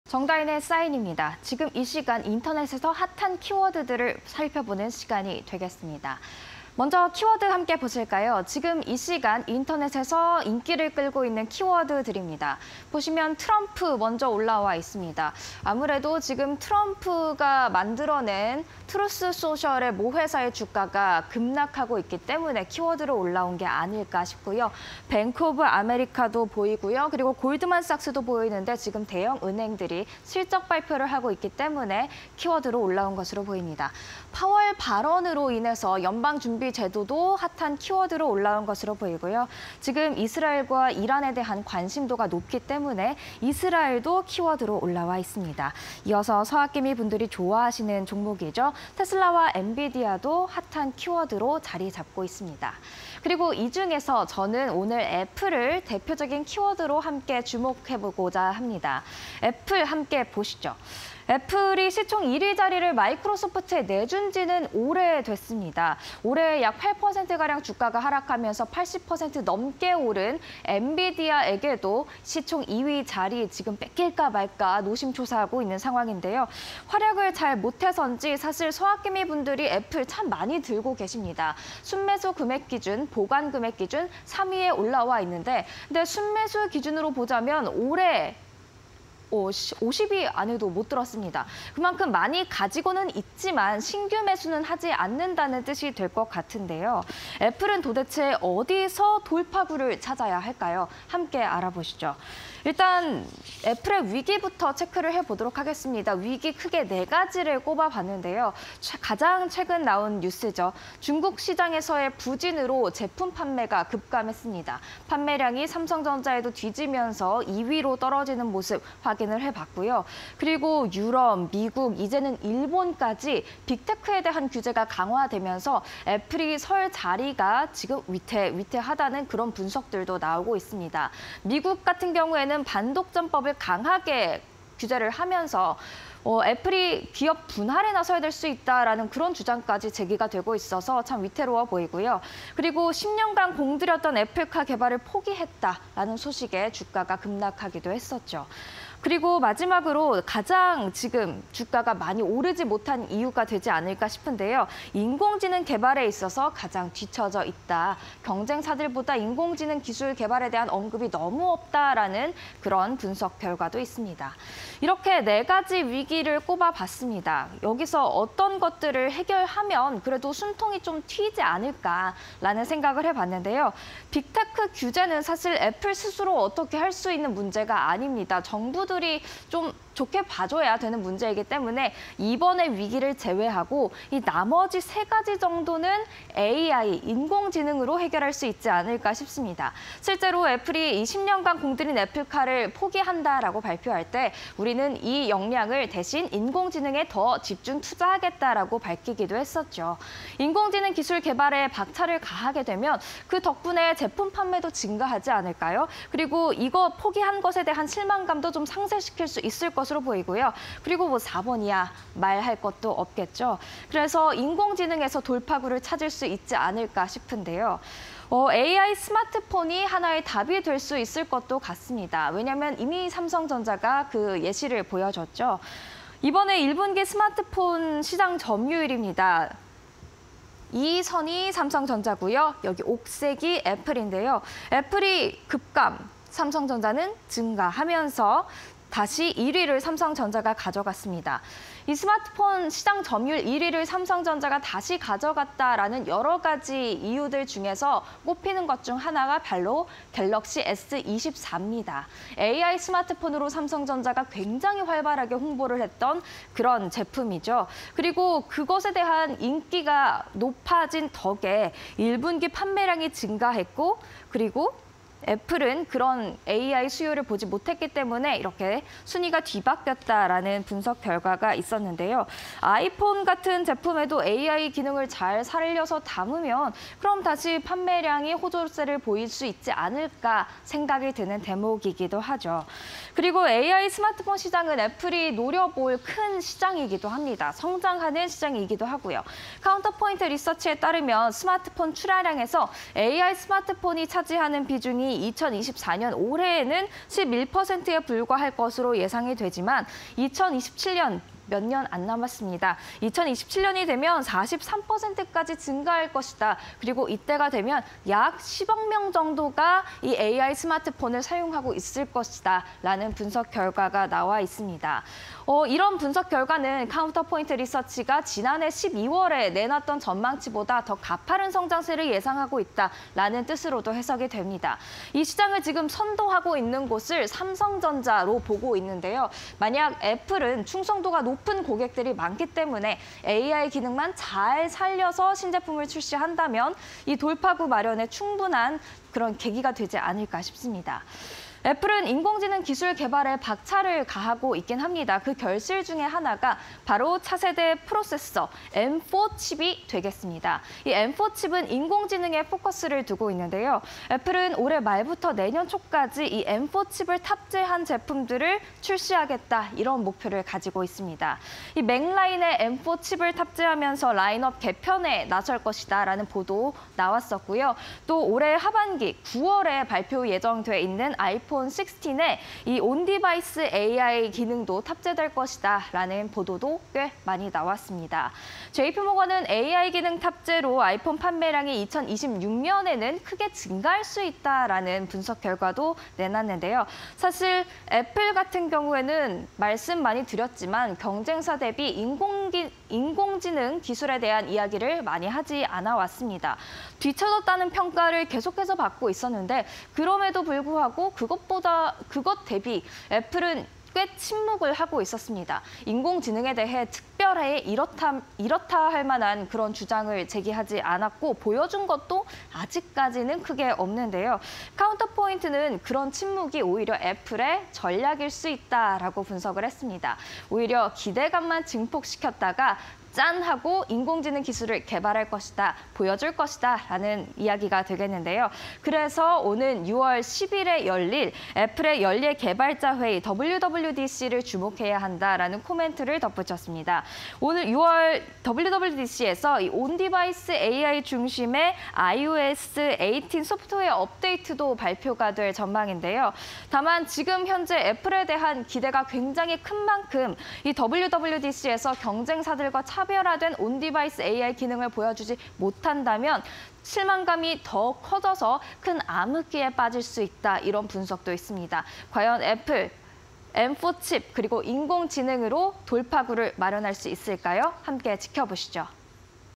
애플의 위기| Market Signal (20240417)
▶  중국 시장에서 제품 판매 부진
▶  빅테크 규제 강화 (유럽, 미국, 일본 등)
▶  10년 공들인 애플카 개발 포기
▶  뒤쳐진 인공지능